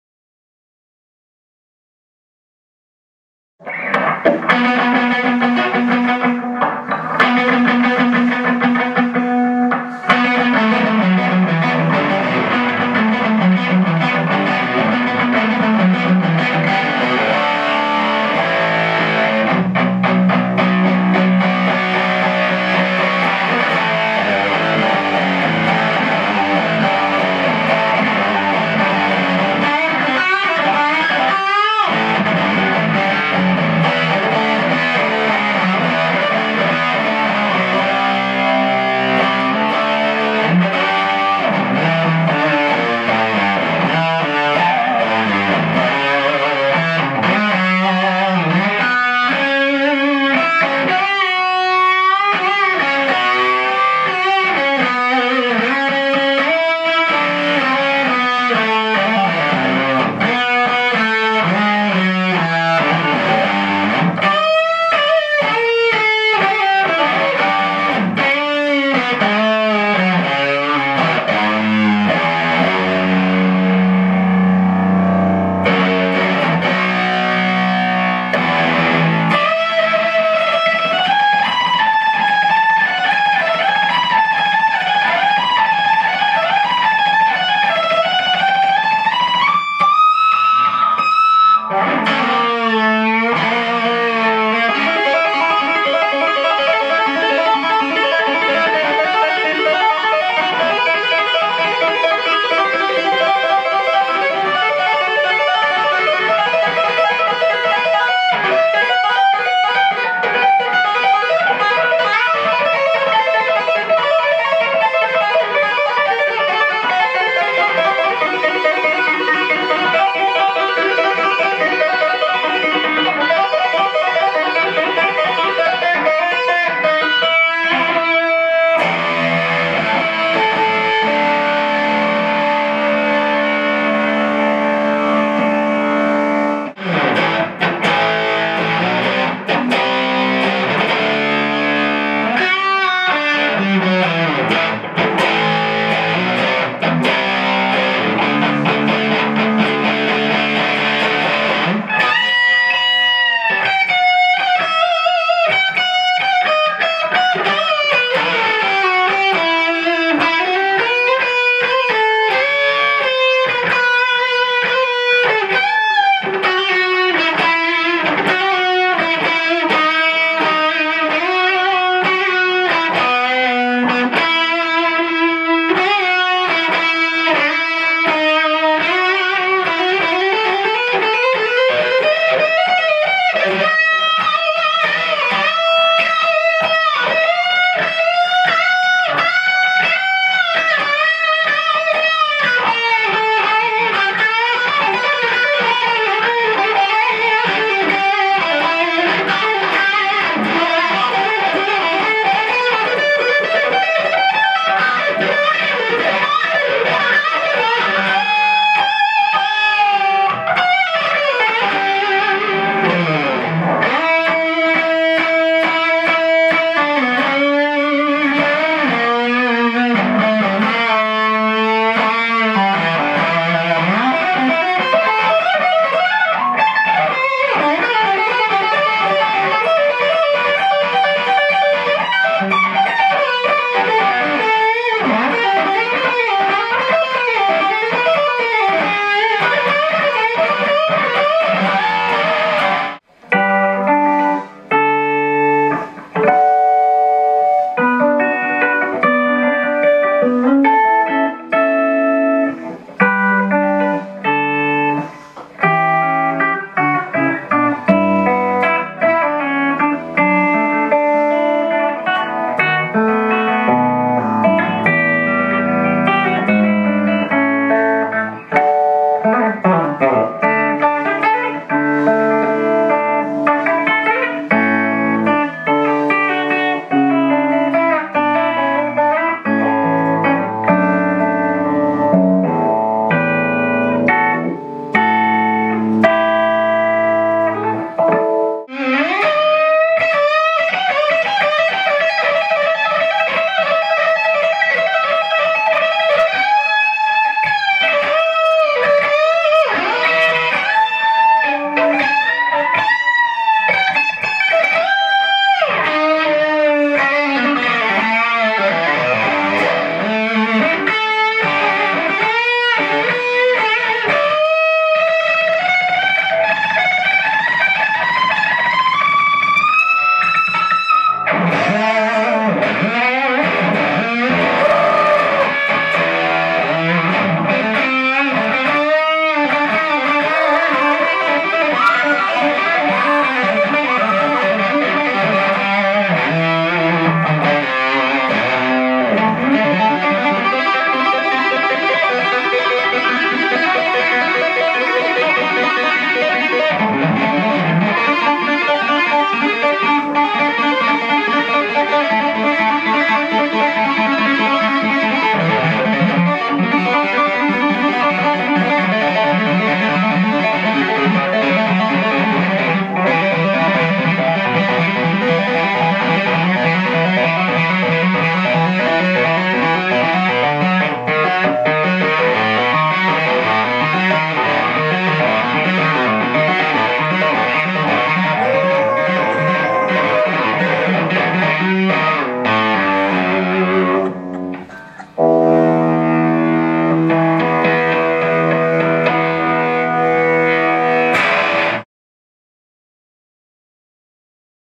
Rock N' Road 2 Audition
Je me représente comme guitariste pour la saison 2 de Rock N' Road. J'ai 18 ans et sa fait 6 ans que je joue de la guitare